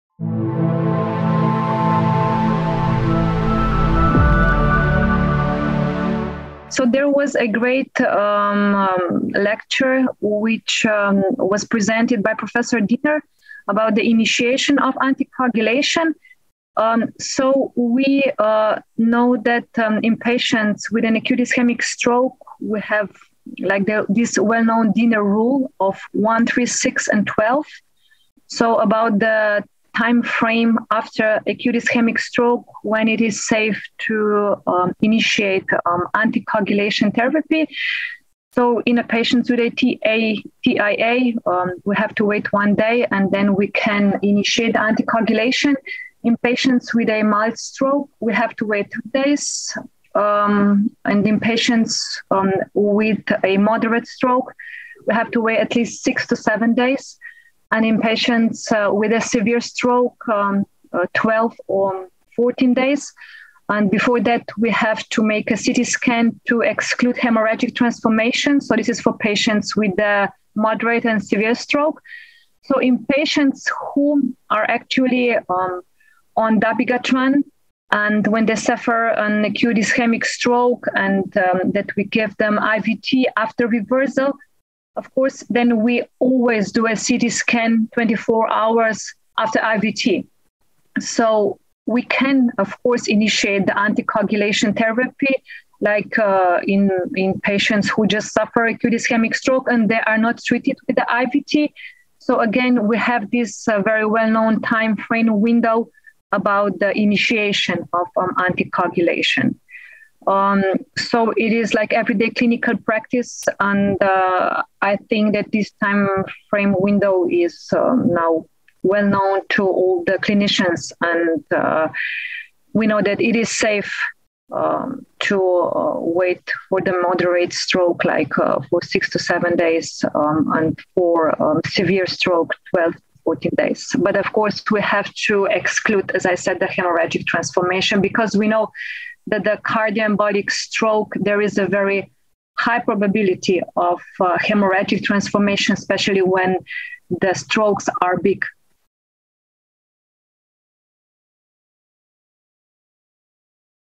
When to start oral anticoagulants after ischemic stroke
Senta Frol, MD, PhD, University Medical Centre Ljubljana, Slovenia, talks on the standard practice guidelines for initiation of anticoagulation after acute ischemic stroke. The “1-3-6-12-day rule” is a well-known consensus, advising increased delay of anticoagulation initiation with increasing stroke severity. For a transient ischemic attack (TIA), anticoagulation can be started after 1 day, day 3 following a mild stroke, day 6 following a moderate stroke, and on day 12 following severe stroke. Dr Frol also highlights the importance of conducting CT before initiation in moderate-severe cases, to rule out hemorrhagic transformation. This interview took place at European Stroke Organisation Conference 2022 in Lyon, France.